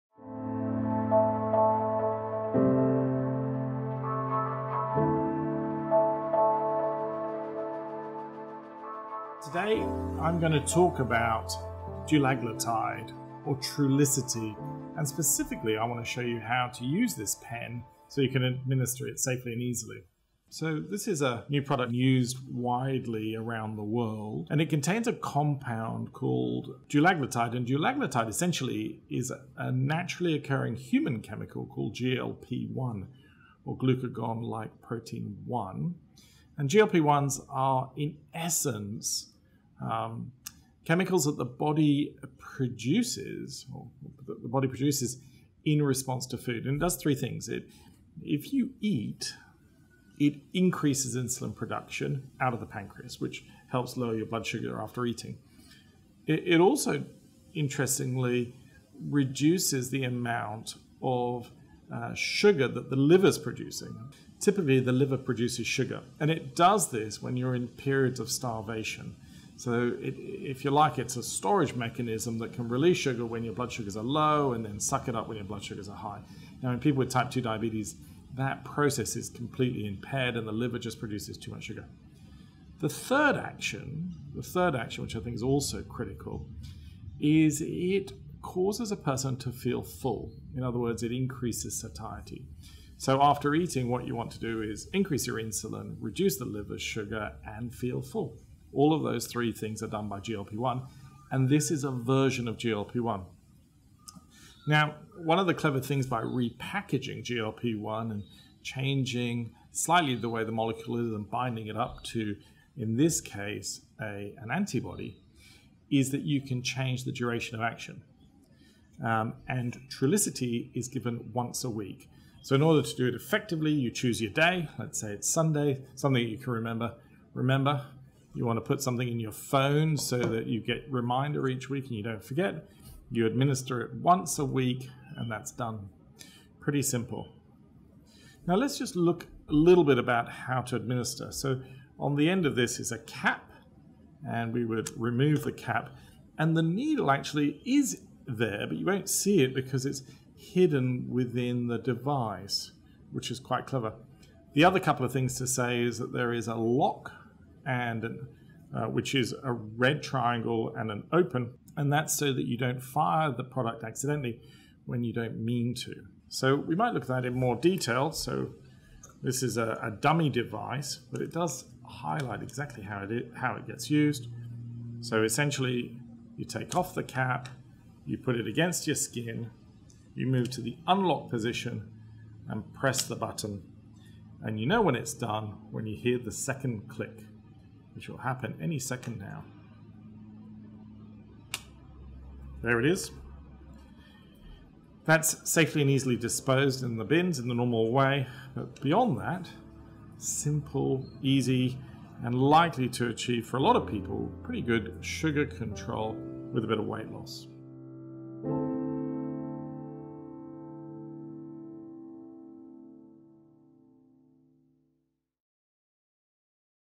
What is Trulicity (Dulaglutide), how does it work, how do I use it?
Trulicity (also known as Dulaglutide) is a treatment for people with Type 2 diabetes.

Today, I'm going to talk about Dulaglutide or Trulicity. And specifically I want to show you how to use this pen so you can administer it safely and easily.
So this is a new product used widely around the world and it contains a compound called Dulaglutide. And Dulaglutide is essentially a naturally occuring human chemical called GLP-1 or Glucagon-like protein 1.
And GLP-1's are in essence chemicals that the body produces in response to food. And it does 3 things, if you eat, it increases insulin production out of the pancreas, which helps lower your blood sugar after eating. It also interestingly reduces the amount of sugar that the liver is producing.
Typically the liver produces sugar, and it does this when you are in periods of starvation. So if you like, it is a storage mechanism that can release sugar when your blood sugars are low and then suck it up when your blood sugars are high. In people with Type 2 diabetes, that process is completely impaired and the liver just produces too much sugar.
The third action, which I think is also critical, is it causes a person to feel full. In other words, it increases satiety.
So after eating, what you want to do is increase your insulin reduce the liver sugar and feel full.
All of those 3 things are done by GLP-1 and this is a version of GLP-1.
One of the clever things about re-packaging GLP-1 and changing slightly the way the molecule is and binding it up to, in this case, an antibody, is that you can change the duration of action. And Trulicity is given once a week.
So in order to do it effectively, you choose a day, let's say it is Sunday, something you can remember. Remember, you want to put something in your phone so that you get a reminder each week and you don't forget.
You administer it once a week, and that's done. Pretty simple.
Now let's just look a little bit about how to administer. On the end of this is a cap, and you would remove the cap. And the needle actually is there but you won't see it because it is hidden within the device, which is quite clever.
The other couple of things to say is that there is a lock which is a red triangle and an open and that is so you don't fire the product accidentally when you don't mean to. So we might look at that in more detail.
This is a dummy device, but it does highlight exactly how it gets used.
So essentially you take off the cap, you put it against your skin, you move to the unlock position, and press the button. And you know when it's done when you hear the second click. Which will happen any second now. There it is.
That is safely and easily disposed of in the bins in the normal way
But beyond that simple, easy and likely to achieve for a lot of people pretty good sugar control with a bit of weight loss.